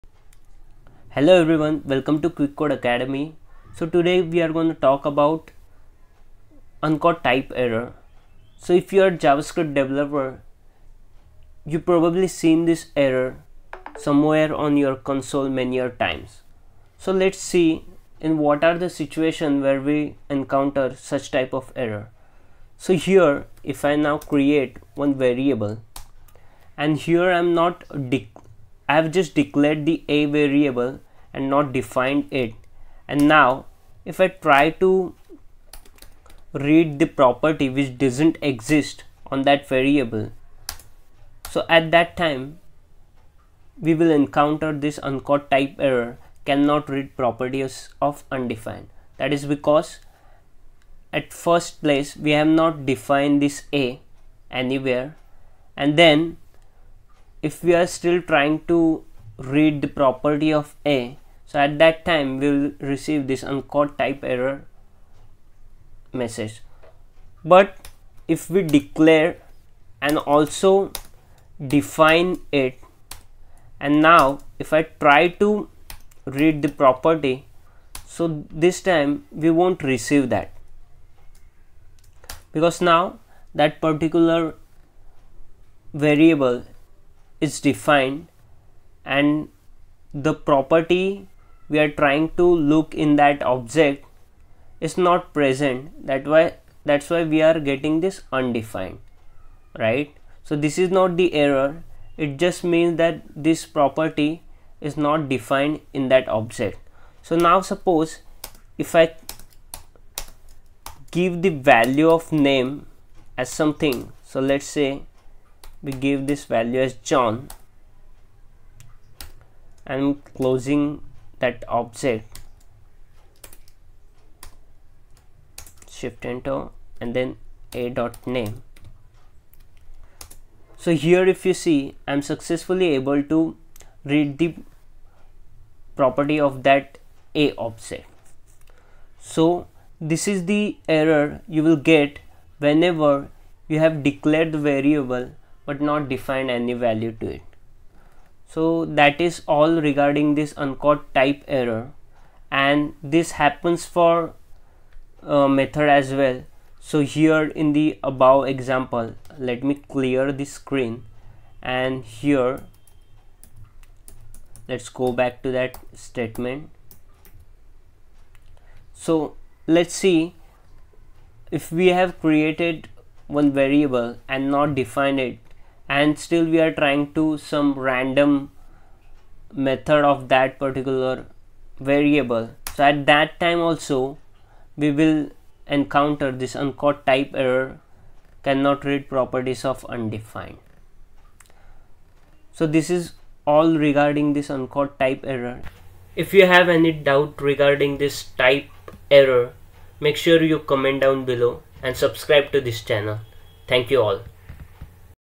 How to Fix Uncaught TypeError: Cannot read properties of undefined
Uncaught TypeError: Cannot read property
If you’re a JavaScript developer, you’ve probably seen this error more than you care to admit. This one occurs in Chrome when you read a property or call a method on an undefined object. You can test this very easily in the Chrome Developer Console.
cannot read property of undefined
✅ Solved: ERROR TypeError: Cannot read property title of undefined